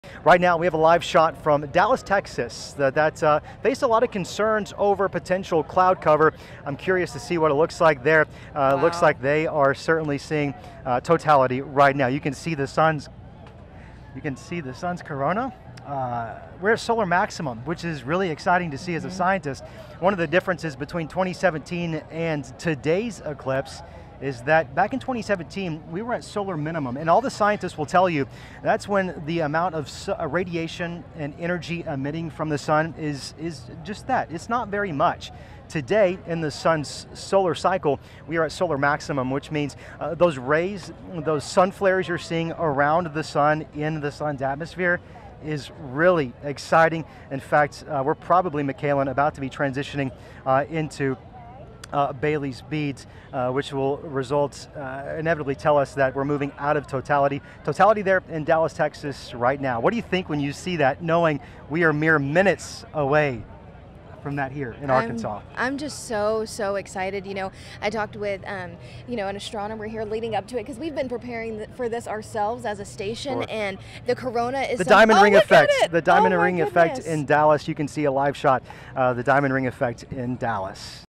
Diamond ring effect during solar eclipse in Dallas, Texas
Here's a look at the total solar eclipse's diamond ring effect that was observed in Dallas, Texas.